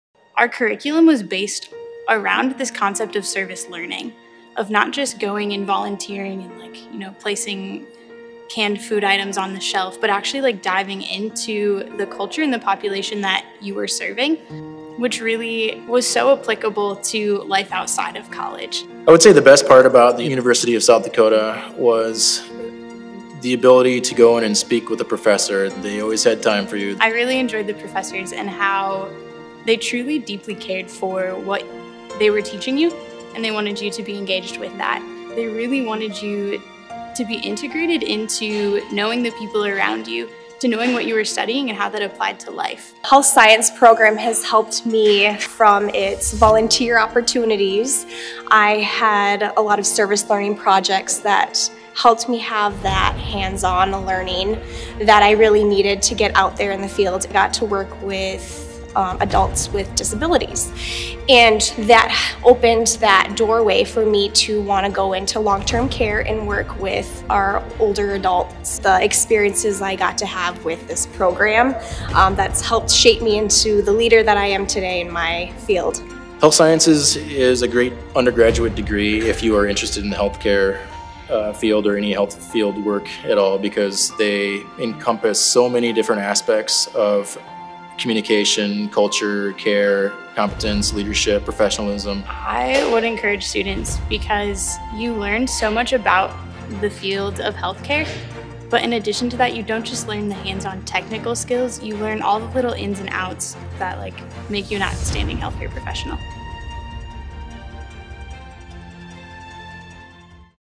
HEALTH SCIENCES | University of South Dakota
With a curriculum based on service learning, Health Sciences programs at the University of South Dakota dive directly into hands on experience ensuring student success in life beyond the classroom. 🐾